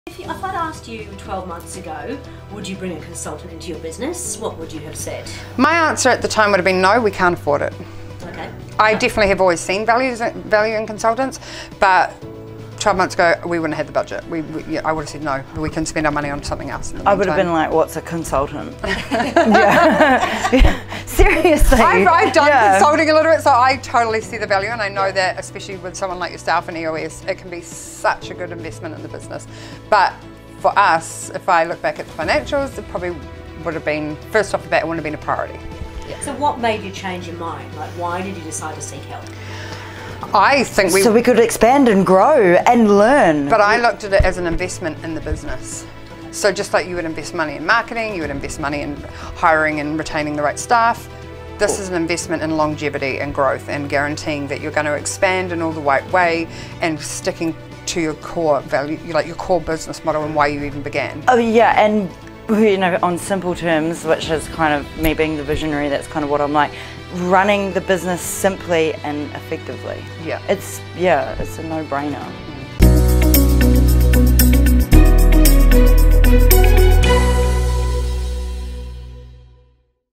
Why Would You Bring In A Consultant | Professional EOS Implementer |Business Traction |Debra Chantry
✅ It’s an investment in the business.

Looking back over the last week at some sporting triumphs (and some lousy draws), these teams and players all had one thing in common.

They had a coach.

How many top performing teams that shine in their respective sports would ever consider not having a coach?

It would be unheard of, right?

🏉 Rugby
🎾 Tennis
🏉 NRL
🏐 Netball
⛳ Golf

It’s the same in business and in today’s video with Nat and Candice, I asked them why they changed their mind in working with me as their EOS coach.

What are your goals for the rest of 2020?
𝐻𝑜𝓌 𝒶𝓇𝑒 𝓎𝑜𝓊 𝓅𝓁𝒶𝓃𝓃𝒾𝓃𝑔 𝓉𝑜 𝑔𝑒𝓉 𝓉𝒽𝑒𝓇𝑒?